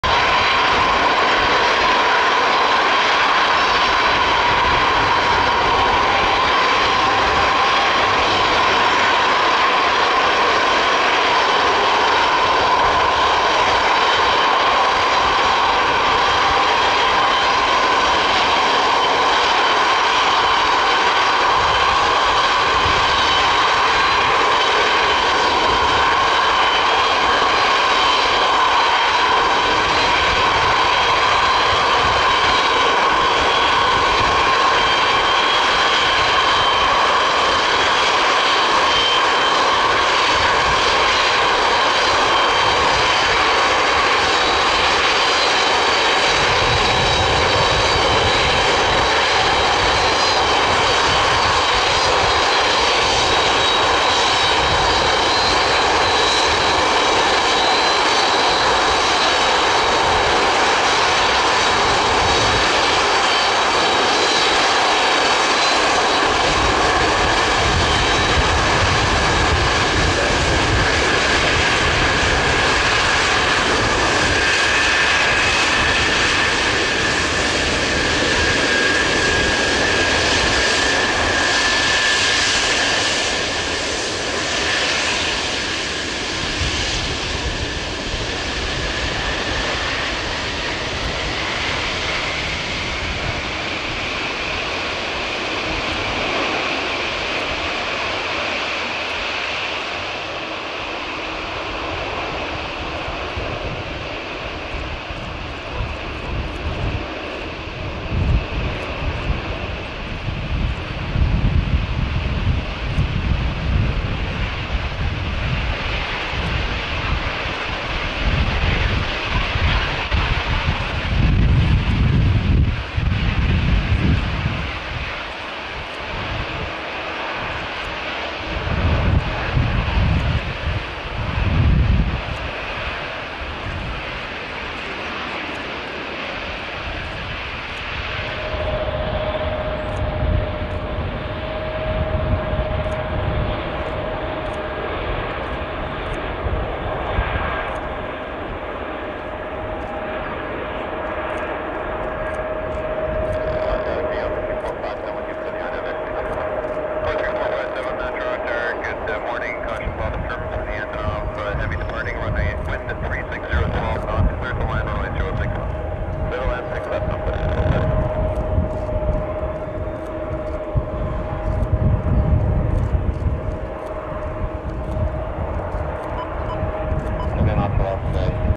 World's largest plane take off | Destroyed during the Russian invasion of Ukraine
The world's largest plane, the Antonov AN-225, has been destroyed during the Russian invasion of Ukraine, according to Ukrainian officials, generating alarm and sadness among the aviation world in which it occupies almost cult status.

The enormous aircraft, named "Mriya," or "dream" in Ukrainian, was parked at an airfield near Kyiv when it was attacked by "Russian occupants," Ukrainian authorities said, adding that they would rebuild the plane.